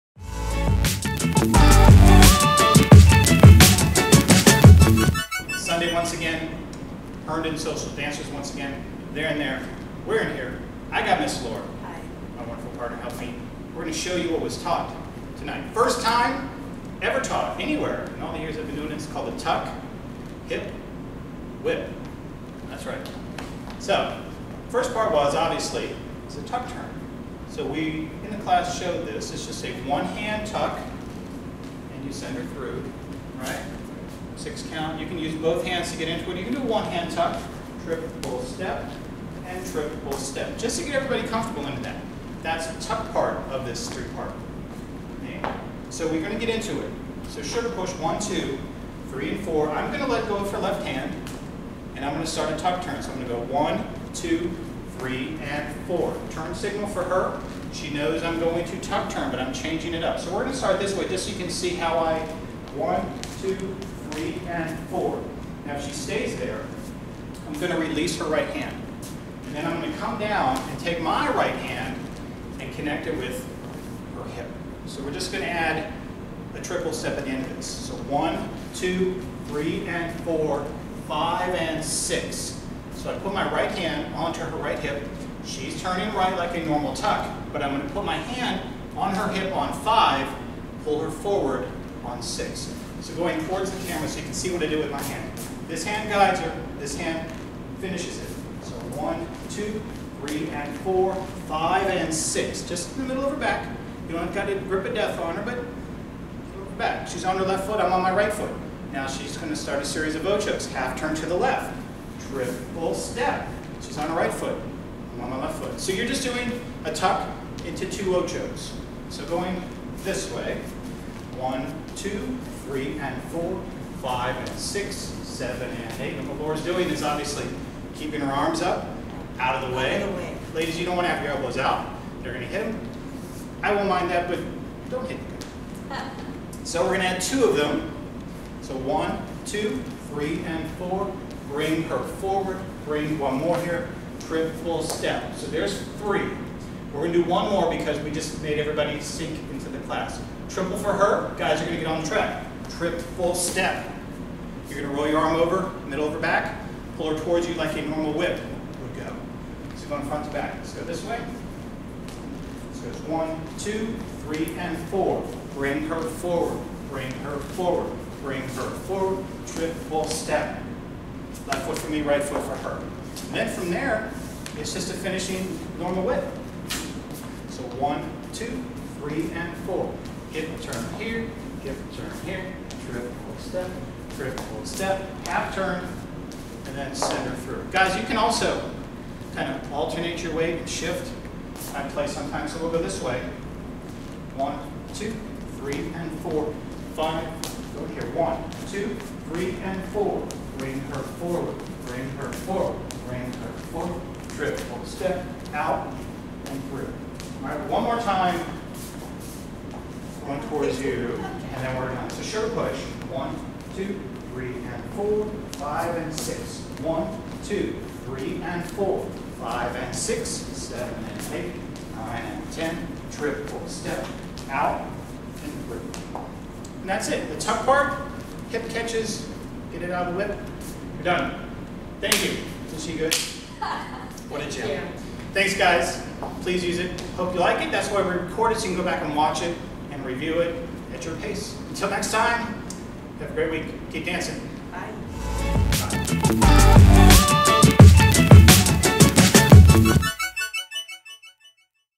Herndon Social Dancers - 7-23-17 - WCS - The Tuck Hip Whip
This was the video workshop review of the West Coast Swing Dance lesson I taught for The Herndon Social Dancers.   Laura and I broke down the sequence so even if you couldn't make the class, you could still watch this video and practice it at your own pace.   :)

Our next dance is on August 6th!  See you then!    :)